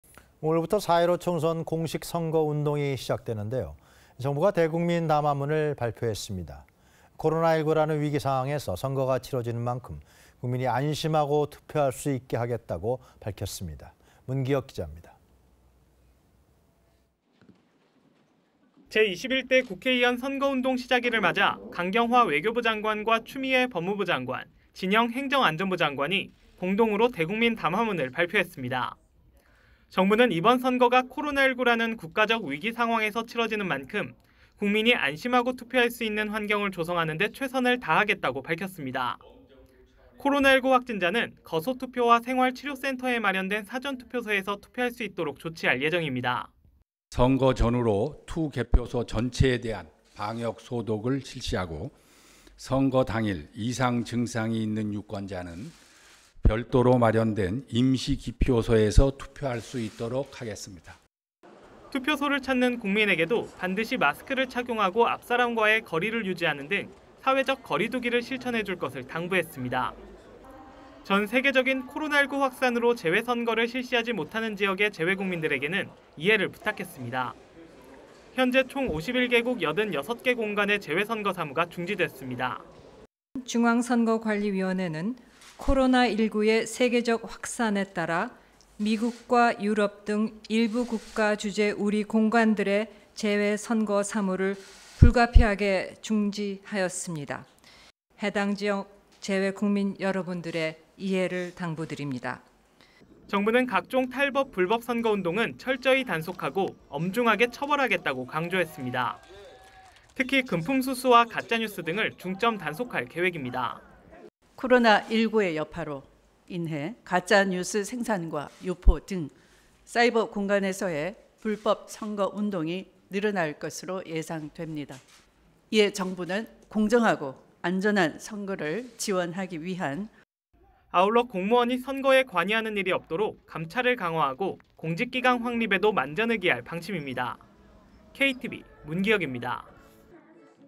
"위기상황서 선거…안전한 투표환경 조성"
4.15 총선
공식선거운동
시작일을 맞아
정부가 대국민담화문을
발표했습니다.

정부는
코로나19라는
국가적 위기 상황에서
선거가 치러지는 만큼,
국민이 안심하고
투표할 수 있는
환경을 조성하겠다고
밝혔습니다.